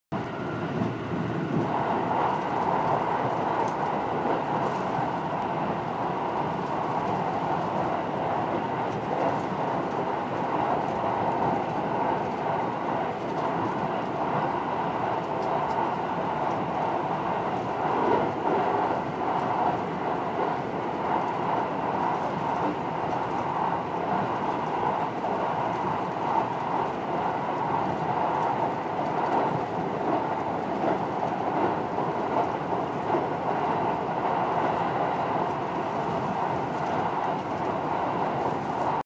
Osaka via Haruka train
going through osaka via haruka express densha